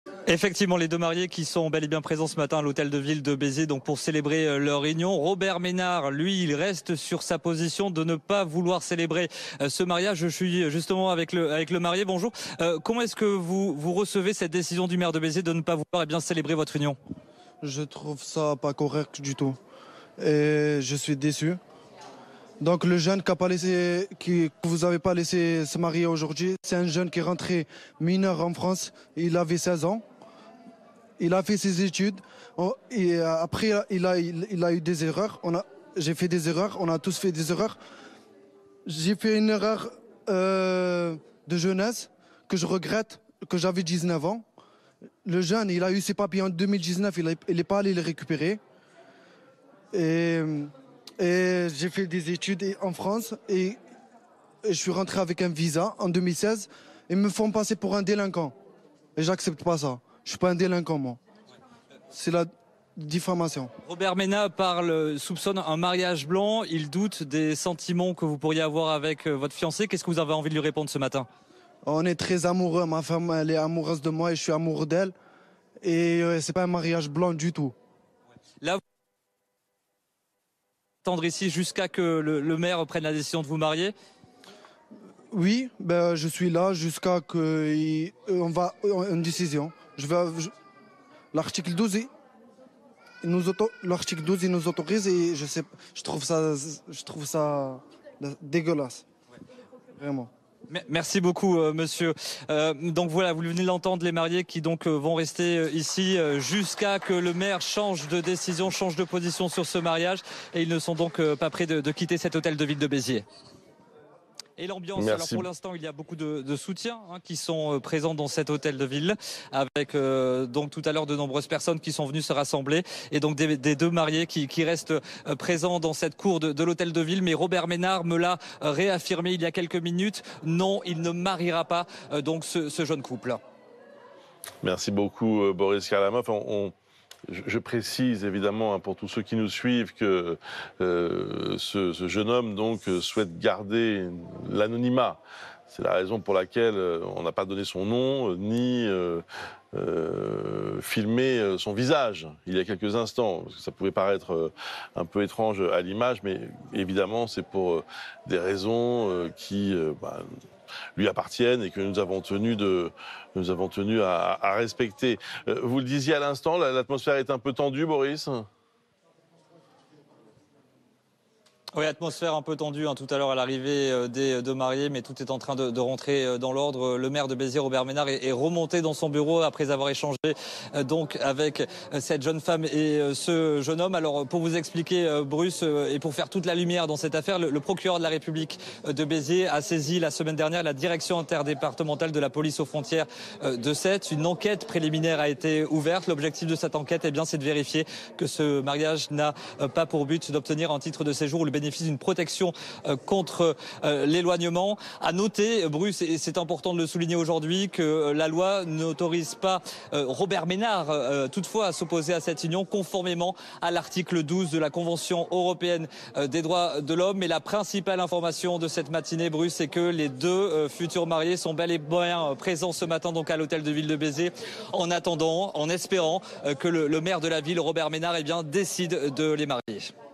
L'Algérien, que Robert Ménard refuse de marier à une Française à Béziers, s'exprime sur BFMTV
Le maire de Béziers refuse de marier vendredi une Française de 29 ans à un jeune Algérien de 23 ans, malgré l'aval de la justice, car il les soupçonne de vouloir procéder à "un mariage blanc". Si l'homme en question est en situation illégale sur le territoire, son mariage avec cette jeune Française n'est à ce stade pas interdit par la justice. Il témoigne au micro de BFMTV.